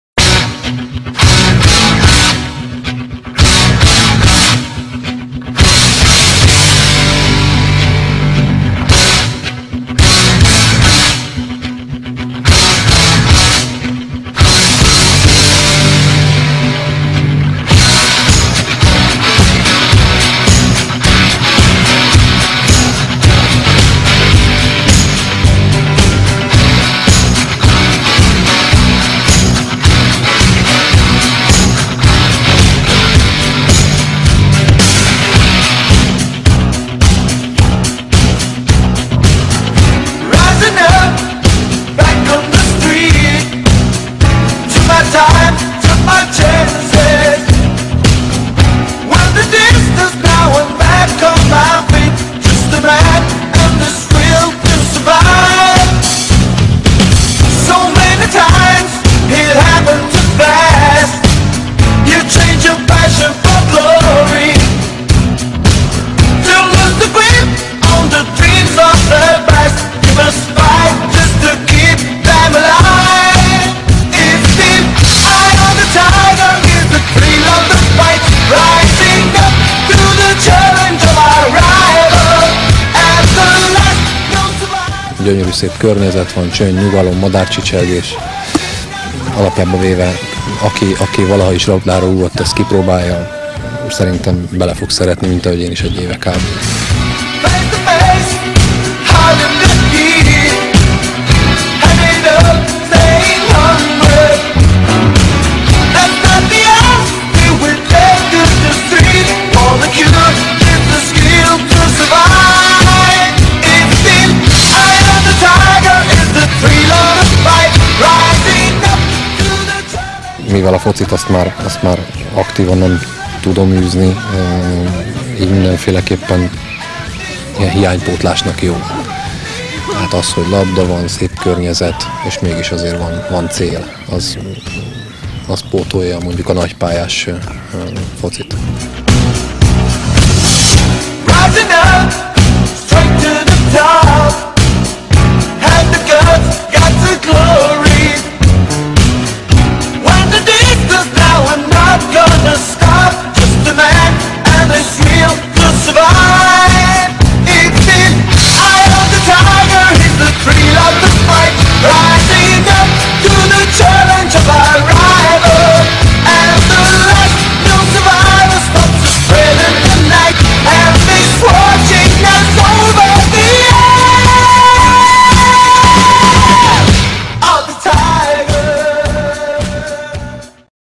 FootGolf World Cup 2012.mp4
L'AIFG - Associazione Italiana FootGolf ha partecipato alla 1° Edizione della FootGolf World Cup, Budapest 1-3 Giugno 2012.